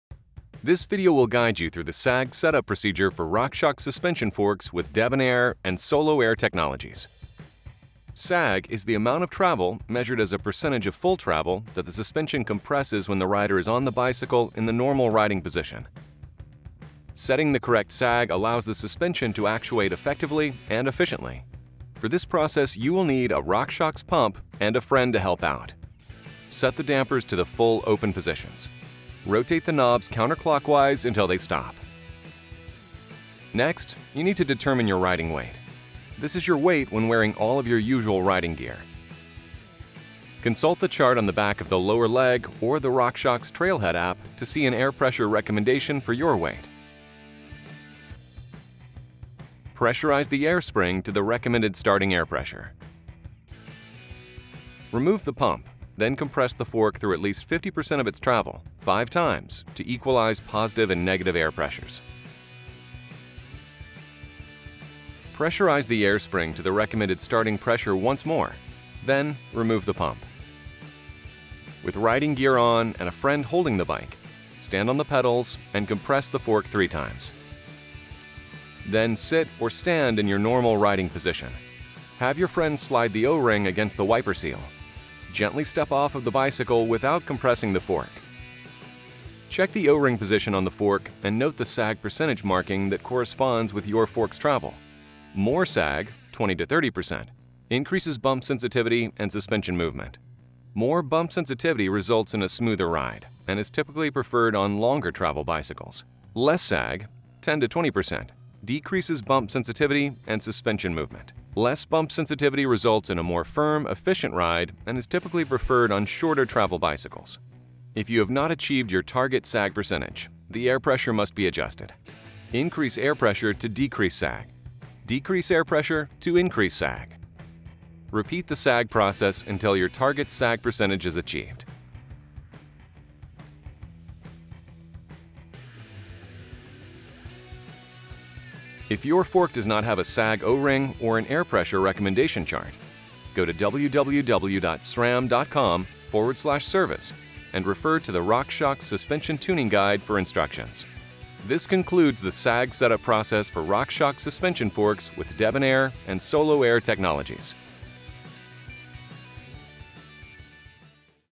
ROCKSHOX: Fork Sag Setup
This video will guide you through the sag setup procedure for RockShox suspension forks with DebonAir and SoloAir technologies.

If you have questions about the installation or service of these or any SRAM components, please consult your local qualified bicycle mechanic.